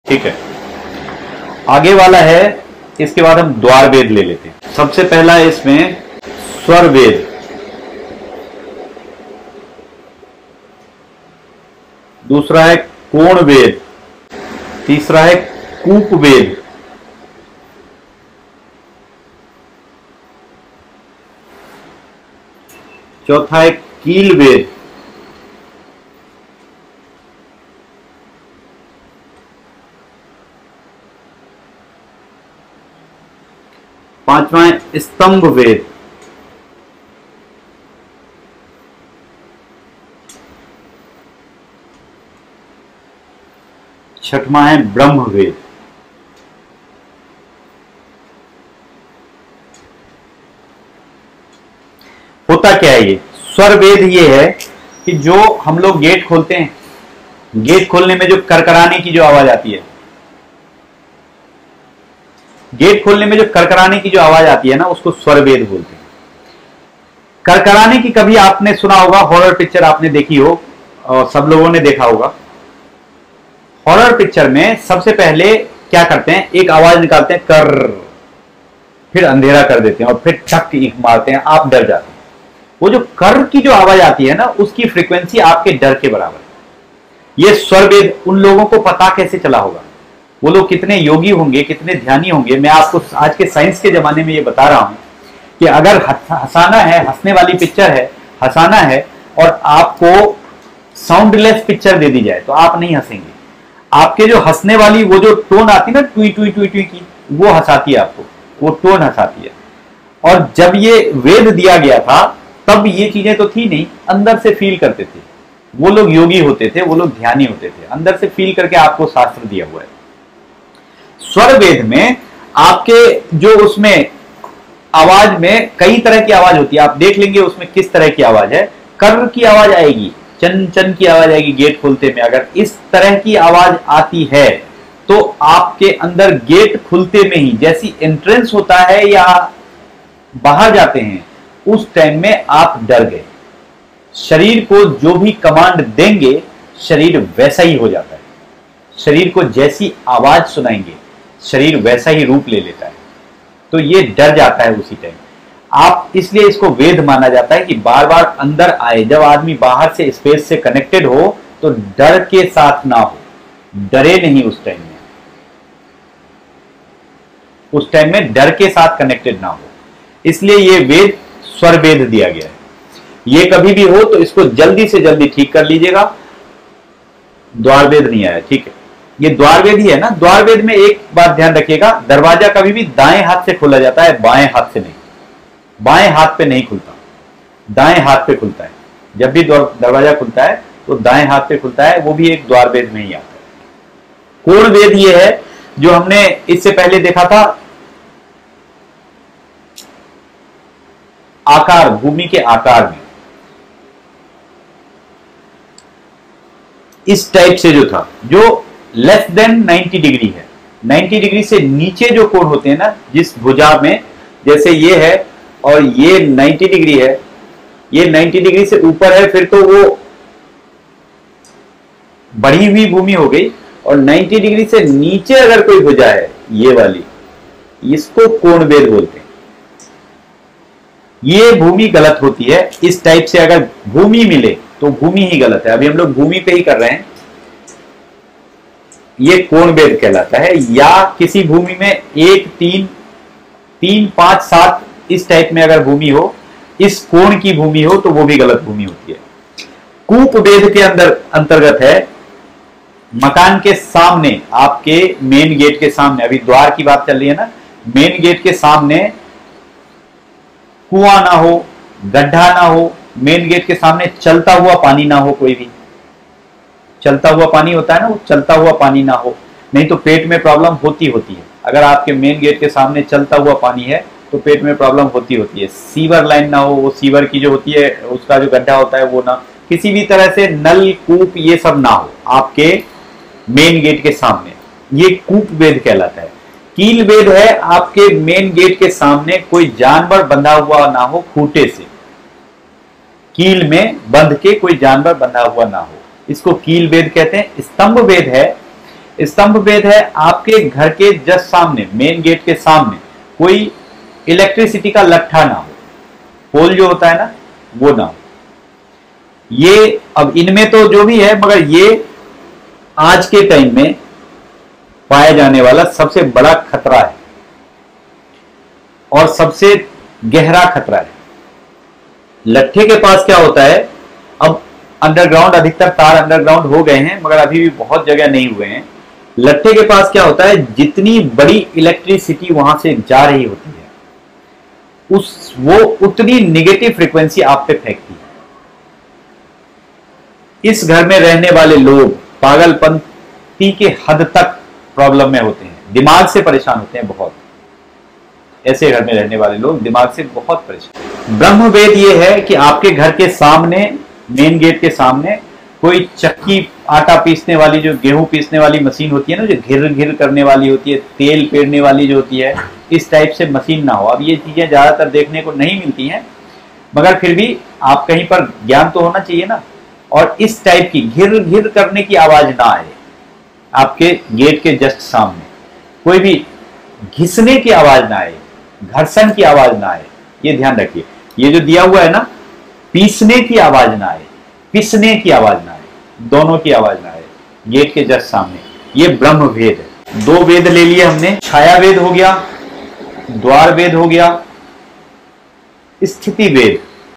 Vastu Learning Part-18 | वास्तु में द्वार वेध | द्वार वेध का महत्व | द्वार वेध के प्रकार
Namaste ,
I am Swami Vimal Jha, Have more than 27 years experience of deep Meditation & Occult Science.

*In this Video I have :-
Described about Vedh and it's one type named Dwar Vedh of Vastu. Dwar Vedh is of 6 types which are described in the video.